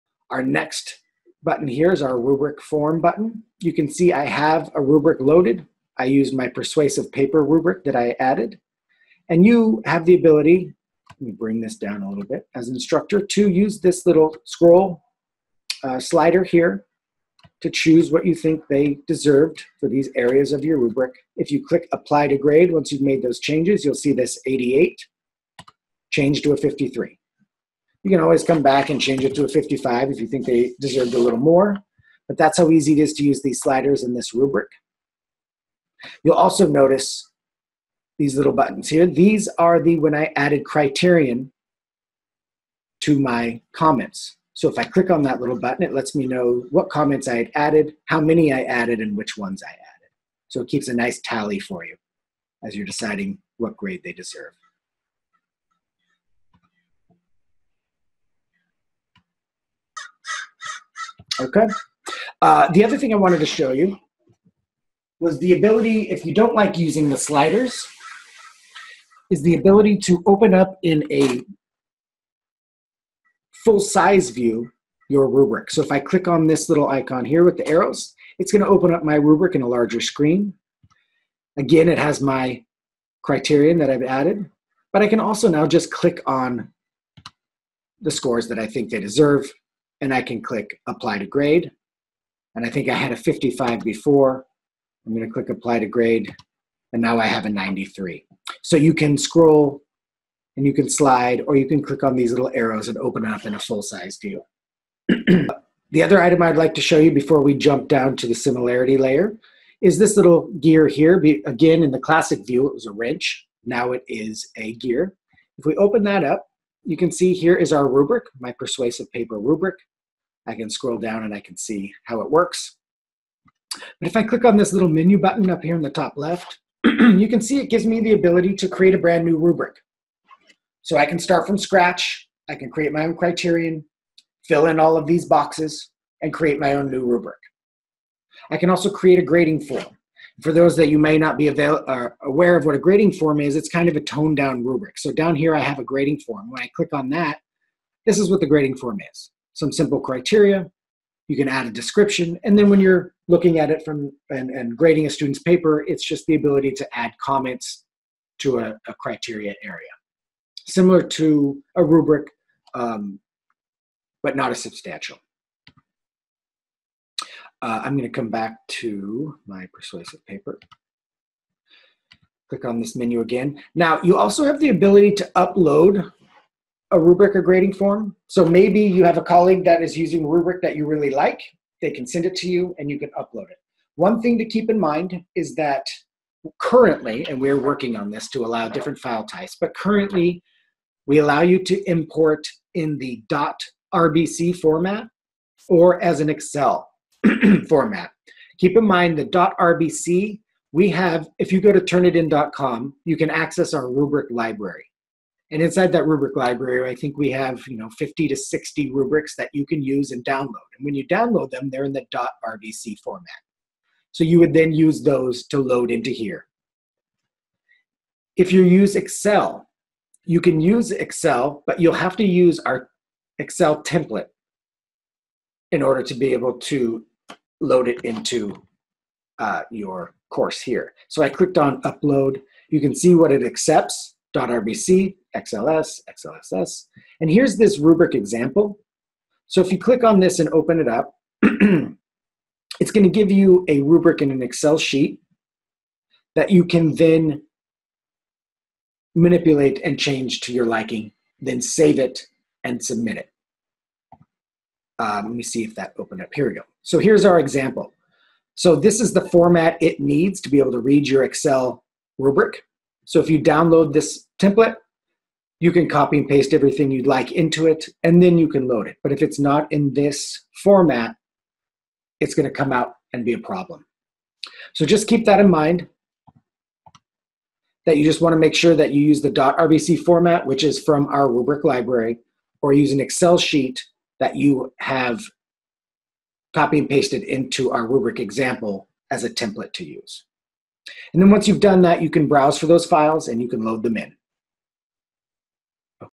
Rubrics in Feedback Studio: Turnitin Demo with Ryan Miller
Demonstration of the Turnitin Feedback Studio grading interface (to be rolled out August 1st, 2017) by Turnitin Customer Success Consultant, Ryan Miller.

Visit our Turnitin eHelp tutorials to take advantage of their anti-plagiarism tools in your own courses